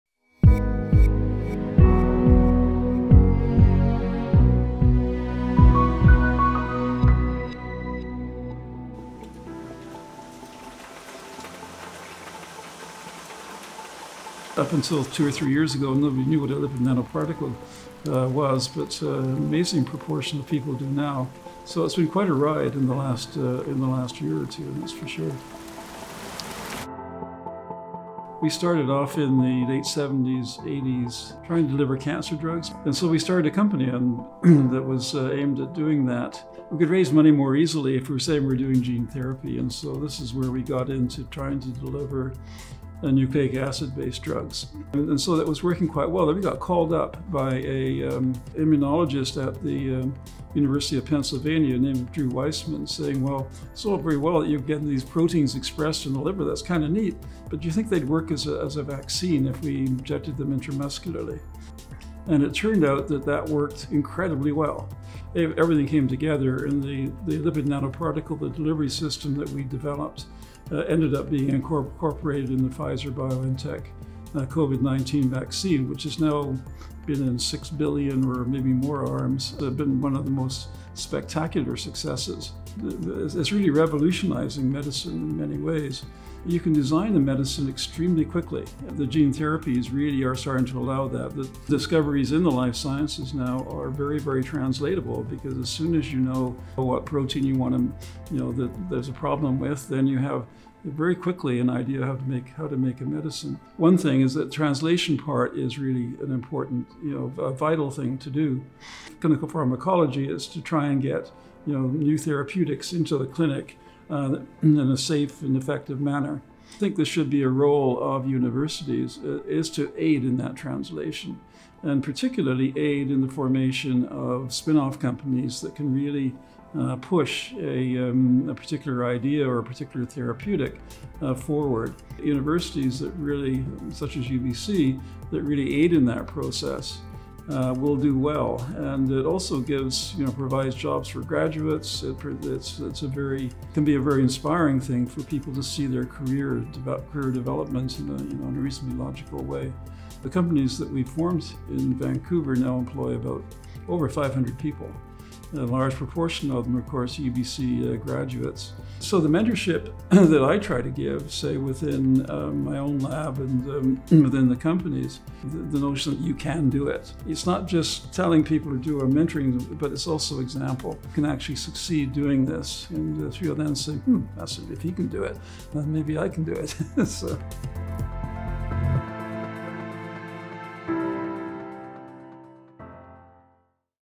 Dr. Pieter Cullis awarded Sultan Karim Medal of Excellence in Clinical Pharmacology
Dr. Pieter Cullis from the University of British Columbia has received the Sultan Karim Medal of Excellence in Clinical Pharmacology. This donor-funded award has been established in honour of Dr. Sultan Karim, a pioneer in the field and professor emeritus in the UBC Department of Anesthesiology, Pharmacology and Therapeutics, to promote, recognize, and celebrate a UBC investigator whose efforts are advancing the field of clinical pharmacology.